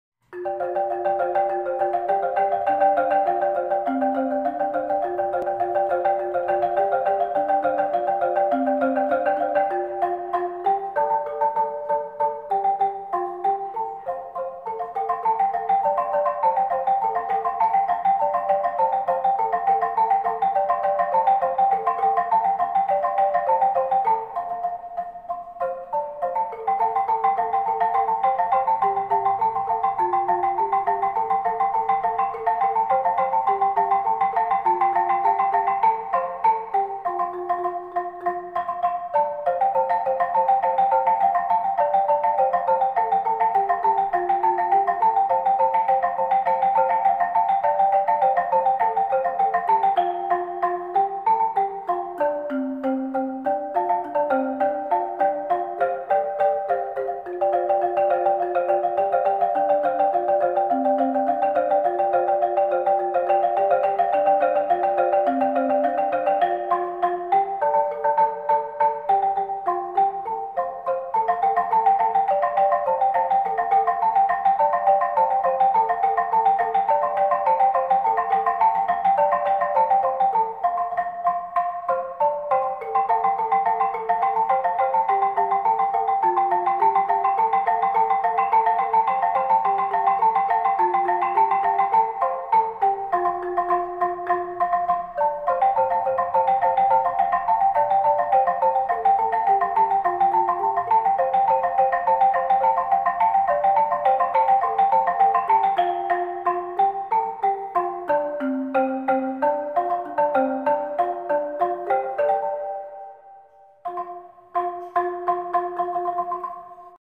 CUNGKLIK BAMBU MUSIC INSTRUMENT || Pentatonic C || Sound Check
Mencoba membuat Cungklik Bamboo Instrument menggunakan bahan bambu tali bahan steiger bangunan yang telah dikeringkan alami selama 6 bulan.
Hasilnya seperti yang kita dengar.